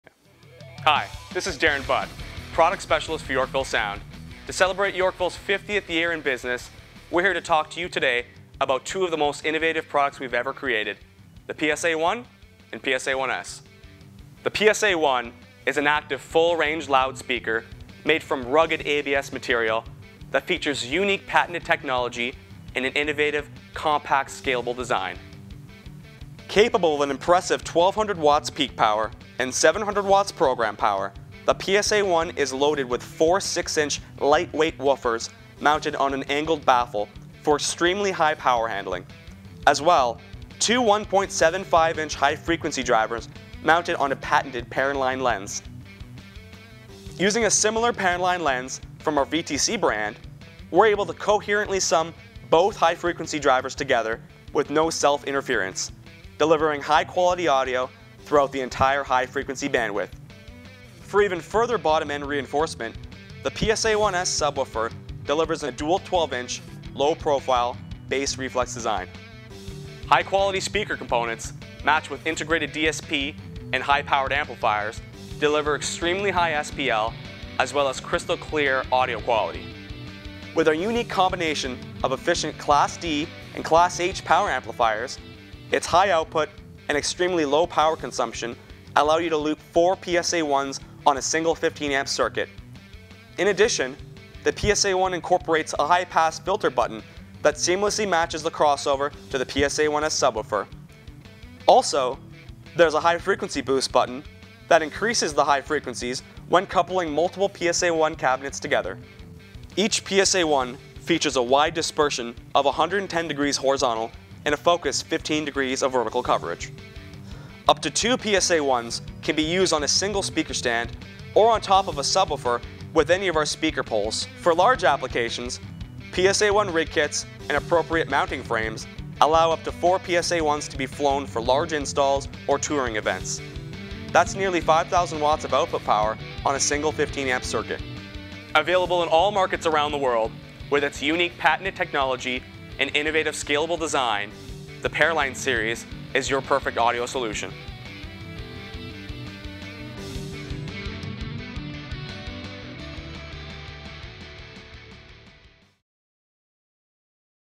Yorkville PSA1/PSA1s
Presented by Darren Butt - Yorkville Product Specialist

Paraline Series
PSA1 - Compact Full Range Active Loudspeaker

Yorkville Sound's Paraline Series active full range loudspeaker system delivers the clarity and precision of a complex line array system in a compact, user friendly format. Innovative cabinet design uses Yorkville Sound's proven Paraline high frequency lens technology pioneered in our flagship VTC Elevation Series line array cabinets.
Two BMS 1.75-inch compression drivers mounted to the patented Paraline lens delivers a focused 15-degree vertical dispersion and wide 110 degree horizontal coverage. Four high quality 18 Sound 6-inch woofers provide a tight and ultra responsive low frequency program.

PSA1 Paraline Lens Technology

The heart of the PSA1 design is the high frequency Para-Line lens. Invented by Tom Danley, this patented technology transforms a point source into a uni-phase plane wave, allowing multiple high frequency drivers to sum coherently in the vertical plane without self-interference. This innovative design provides an effective impedance transformation while at the same time adjusting the path length in a continuously variable way creating a dispersion pattern with the same characteristics as that of a much deeper conventional horn.

The Paraline Lens eliminates the impractical depth needed to make a conventional horn that would sum into a non-interfering source needed for multiple cabinet arrays. In the case of the PSA1, a vertical dispersion pattern of 15° is achieved with a Paraline lens less than one inch deep, where a conventional horn design would need to be several metres in length.

Innovative Multiple Woofer Configuration

Four 6-inch high power woofers mounted on the "V" shaped front baffle /horn utilize the entire front surface area of the cabinet to increase loading on the drivers and control pattern coverage at lower frequencies. This unique four woofer design increases power handling while reducing power compression by spreading the work load out over four long excursion voice coils and heat sinks (magnets).

Designed for Live Sound in the Real World

Rugged injection molded ABS cabinet and Neodymium magnet woofers minimize overall cabinet weight making a full system transport and set-up can easily be handled with a single operator. Designed to deliver maximum performance with minimum complication, the PSA1's integrated power amplifiers and internal DSP mean no external processing is required. Direct connect to mixer source and set system levels, and the system is ready for the show.

Highly efficient amplifier design means each PSA1 draws less than 3 amps in typical operation meaning up to four cabinets can operate safely on a standard 15 amp service. This allows efficient set up using AC loop thru connections on the PSA1 while avoiding nuisance tripping of mains breakers. This also minimizes the need for multiple extension cables or access to multiple house circuits often needed for an active PA setup.

Ideal for mobile PA applications in venues where access to complex trussing or roof hangs isn't practical, the Yorkville PSA1 cabinet mounts quickly and securely on a typical speaker stand. Innovative dual position stand mount offers two potential cabinet angles for expanded setup options. Up to two PSA1 cabinets can be stacked safely and securely on a single traditional stand.

Features

Ultra High Performance Compact Active PA System
Uses Patented Paraline Horn Lens Technology
(from Yorkville Sound's Flagship VTC Elevation Series Vertical Array Touring Systems)
High quality Speaker Components
(including 18 Sound 6-inch Woofers and BMS Compression Drivers)
Focused 15° Vertical Dispersion for Minimum Interference Between Arrayed Cabinets
Wide 110° Horizontal Dispersion Pattern
Manufactured Using Light-weight/Strong ABS Cabinet Material
Stacks using Standard Speaker Stand on Integrated Dual Angle Stand Mount Adaptor
(Maximum Two Cabinets per stand)
Installation Hardware Available
Up to Four PSA1 Cabinets Can Be Easily Powered on one 15 Amp Circuit